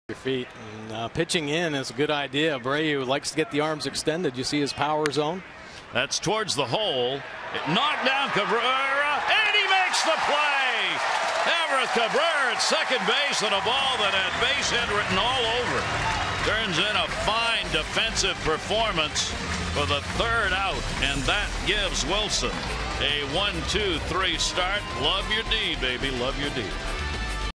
CWS@BAL: Cabrera's slick defense takes away a hit
5/28/15: Everth Cabrera makes the diving stop and fires to first from the seat of his pants to take a hit away from Jose Abreu